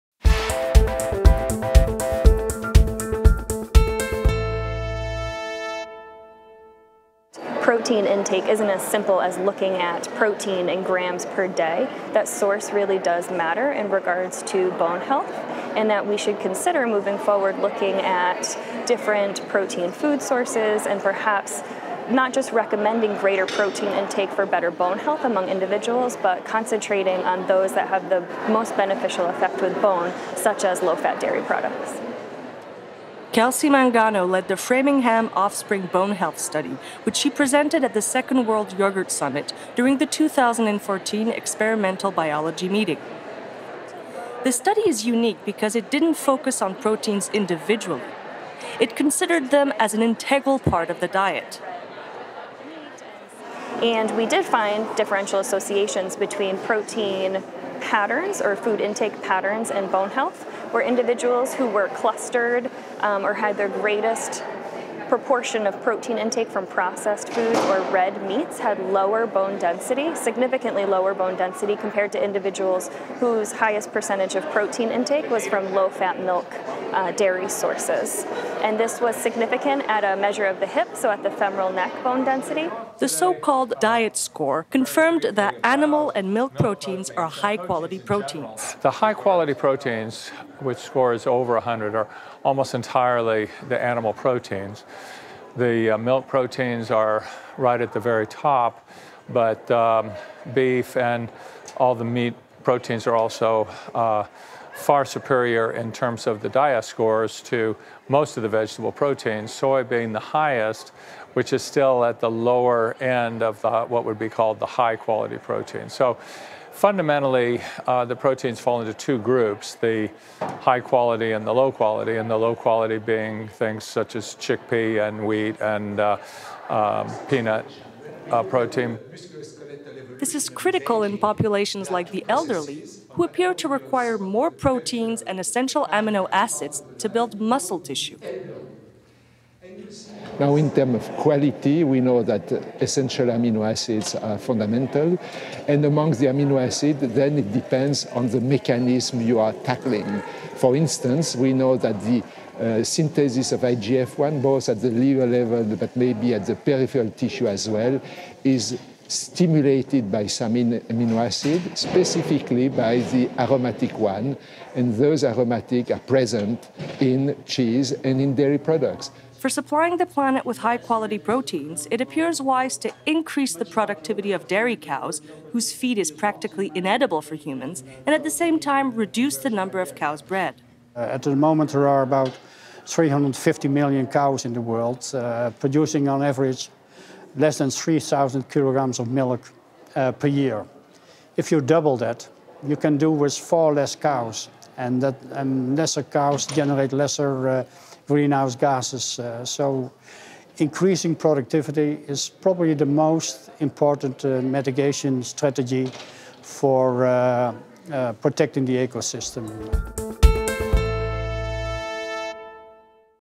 The global challenge of high quality proteins intake in elderly
During the 2nd World Yogurt Summit, which took place during the 2014 Experimental Biology meeting, experts highlighted the importance of a protein-rich diet for the preservation of muscle mass and strength in the elderly

Every year, the Yogurt in Nutrition Initiative for a Balanced Diet brings together scientific experts from all around the world to present recent progress in research on yoghurt and health during the annual Experimental Biology meeting

The 2014 edition drew 250 health and research professionals to San Diego, California, where the Summit took place in partnership with the American Society for Nutrition, the UK Nutrition Society and the Danone Institute Dairy International.

Subtitles in five languages (English, French, Spanish, Portuguese, Chinese)